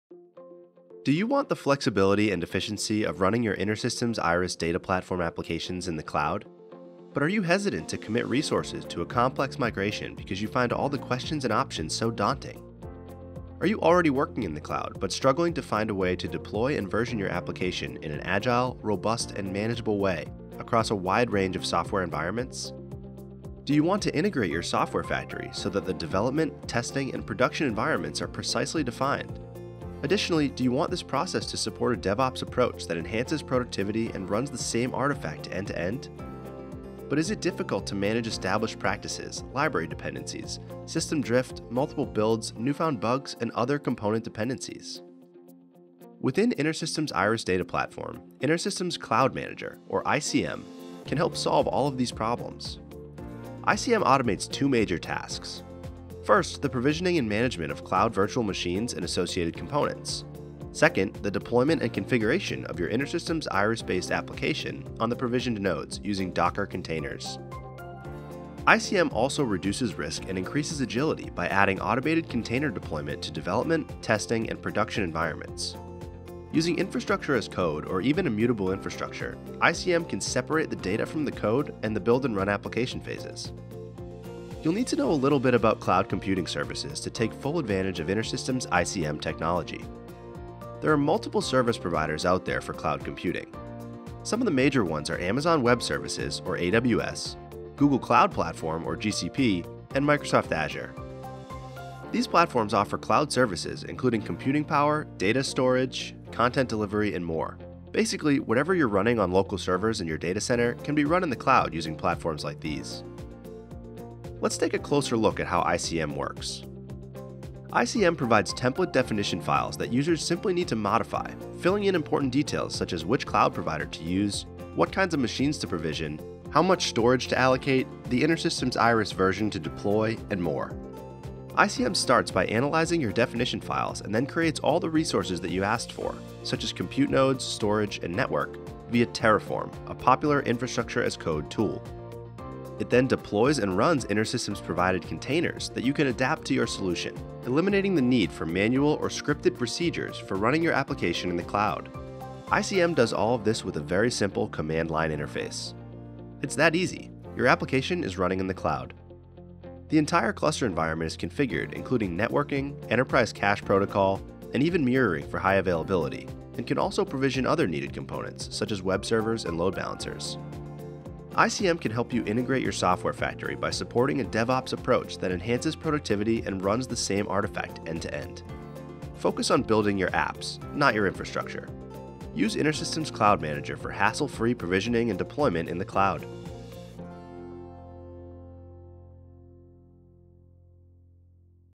What is InterSystems Cloud Manager?
This video provides an introduction to InterSystems Cloud Manager (ICM) and its capabilities.

See the postings regarding developing and deployment InterSystems dataplatform solutions in Cloud